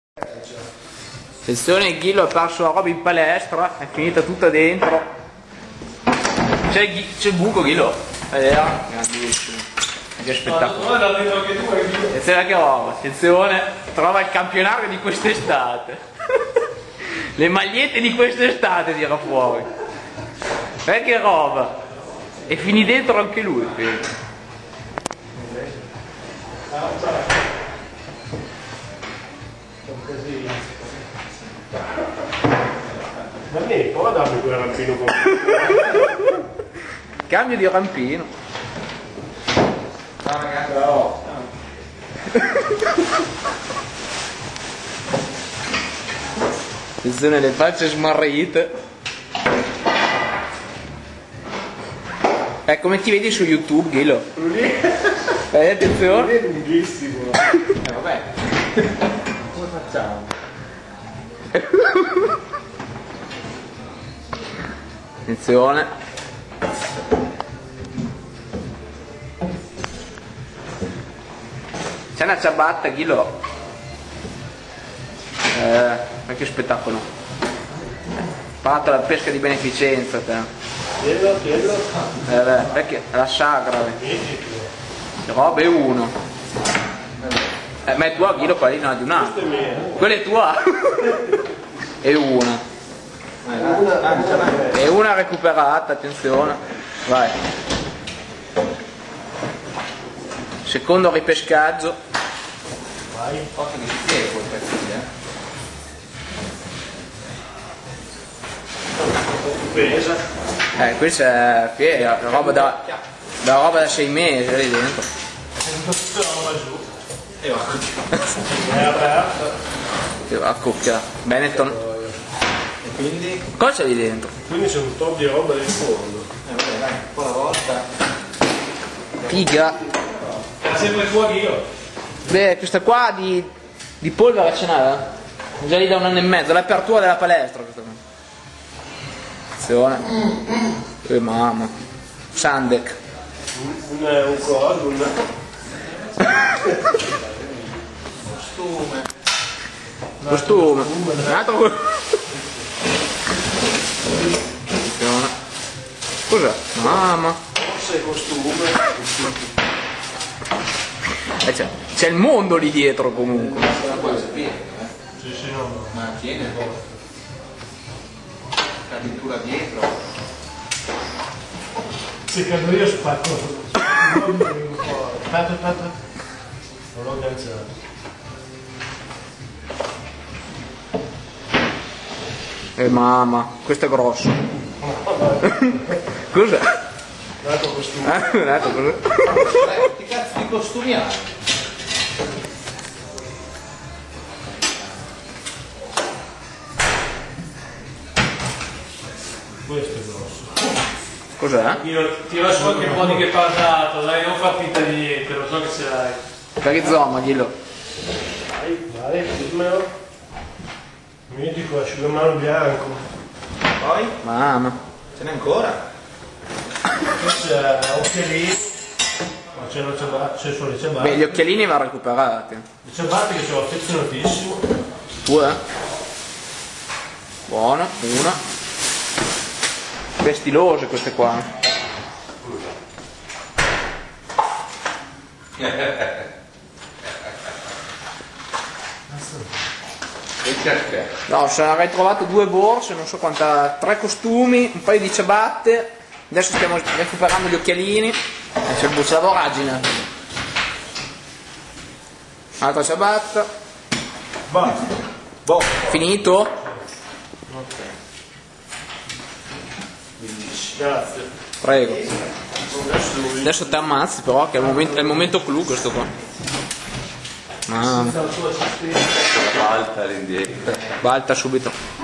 Ghillo e il buco nero negli spogliatoi - Sala Baganza 2/2008
Il Ghillo perde la borsa in una voragine tra gli armadietti degli spogliatoi della palestra. Questo è il filmato del ripescaggio